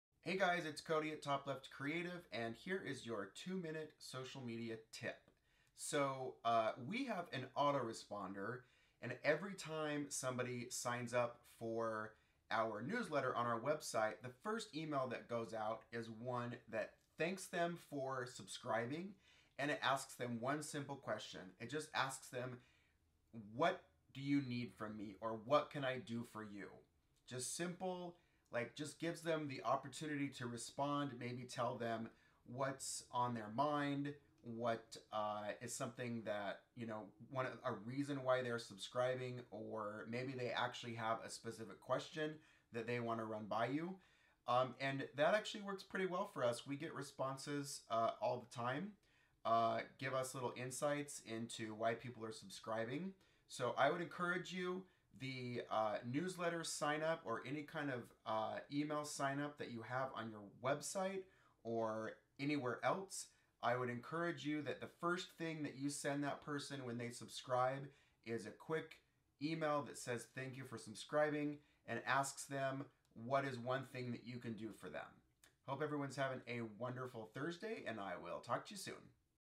Best Real Estate Email Autoresponder Opener
My favorite question to ask:

What made you sign up?

You can also ask:

What’s the one thing I can help you with?

This email in our welcome autoresponder series is so powerful. We often get replies to it that help us understand our customers and also give us a chance to provide real value up front.